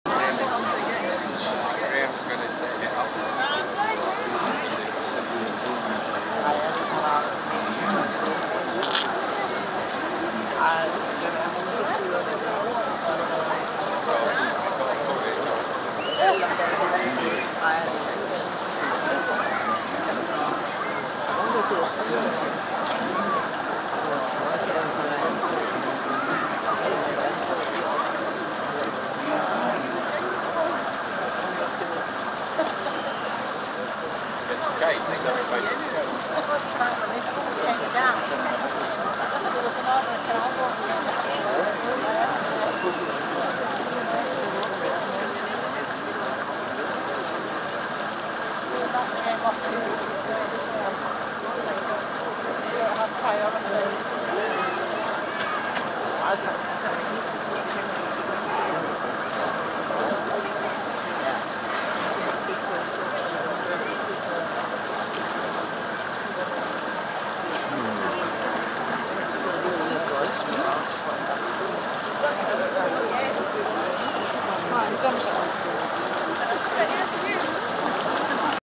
Falkirk Wheel
The Falkirk Wheel. If you are an engineer - or a tech geek by nature - then you should visit the Falkirk Wheel near Edingburgh - Scotland.
Impressive engineering!!